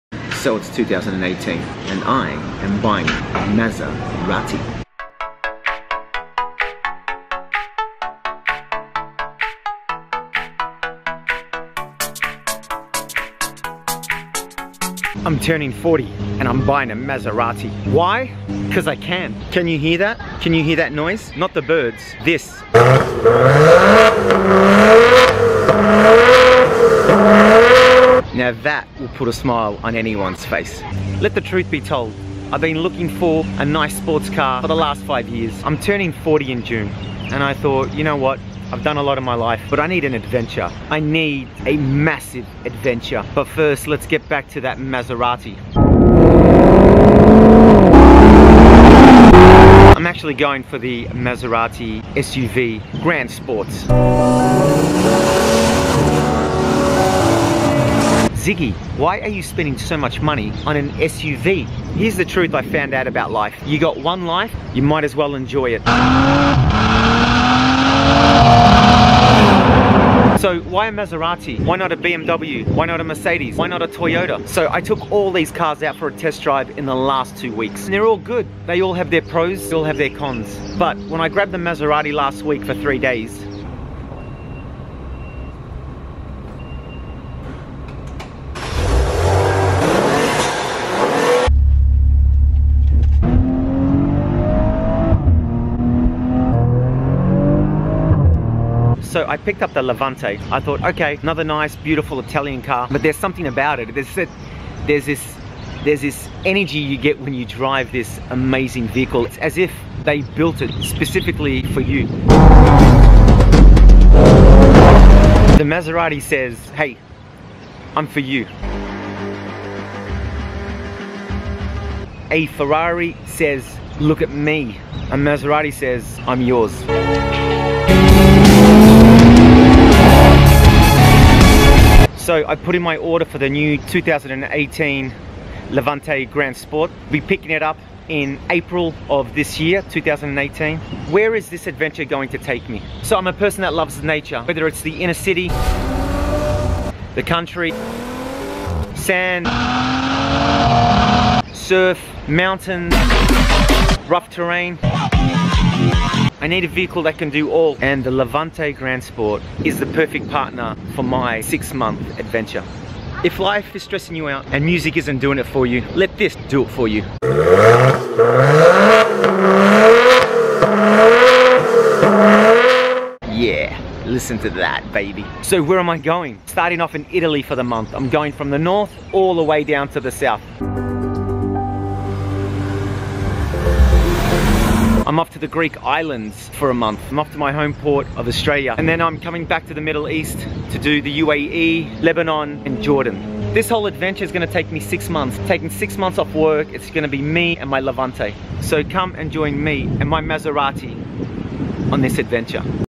Me and My Maserati.
I'm off on a 6 months trip with my new Maserati Levante to Australia, Italy, the Greek Islands, New Zealand and Lebanon!